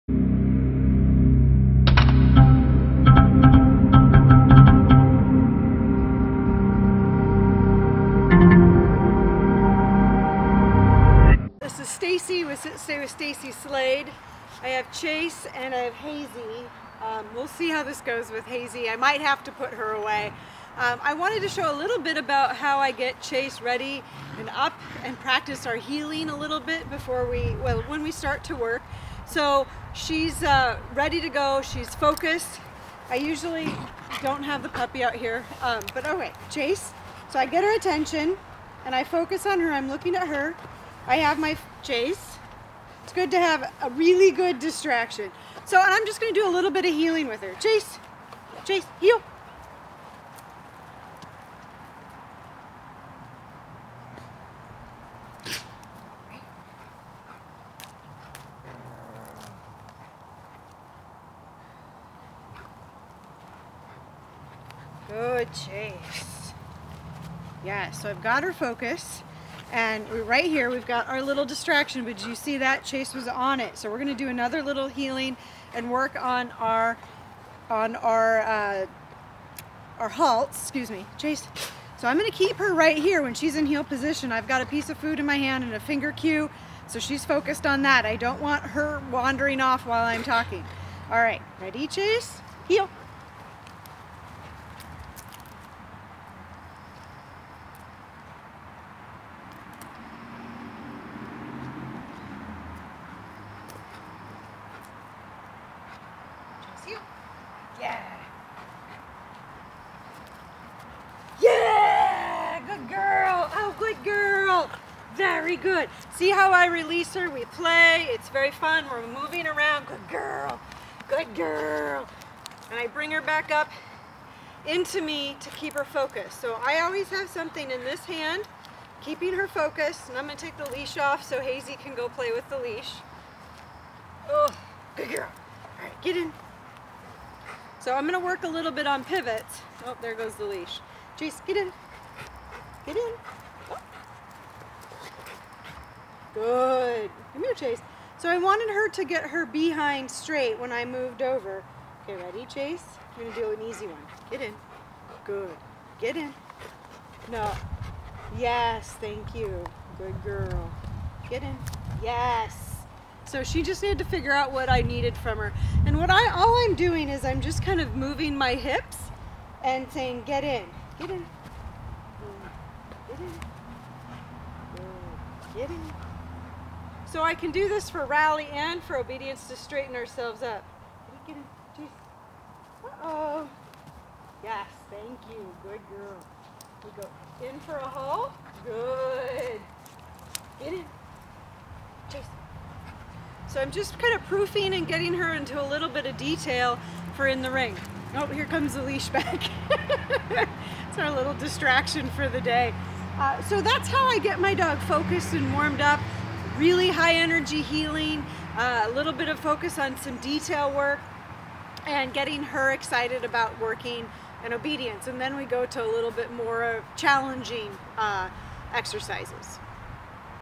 How I warmup my dog for training and get her focused to work (with cute distractions)!
I am warming Chase up on a couple short heeling drills to get her focused and engaged. I keep it short, happy and high energy. Hazy does a great job at being our distraction!